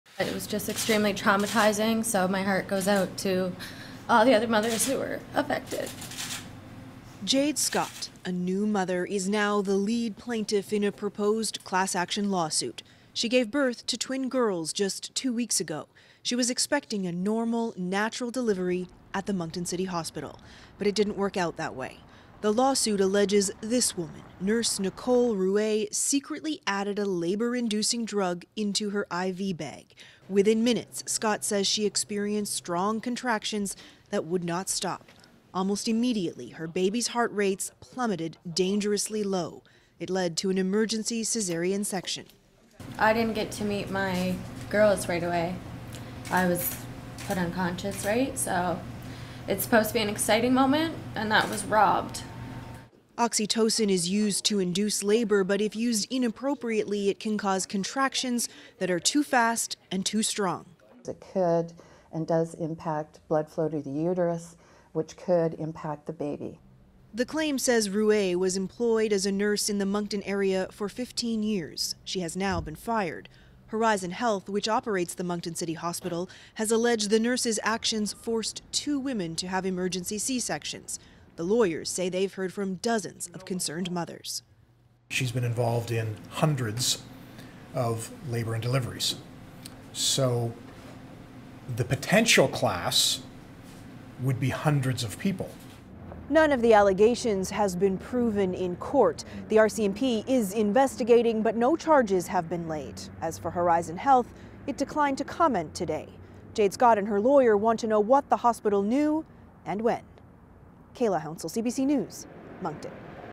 Mother accuses N.B. hospital, nurse of alleged misuse of labour-inducing drug
A proposed class-action lawsuit has been launched against the Horizon Health Network and nurse Nicole Ruest on behalf of women who were allegedly "inappropriately" given a labour-inducing drug at Moncton Hospital and required emergency caesarean sections.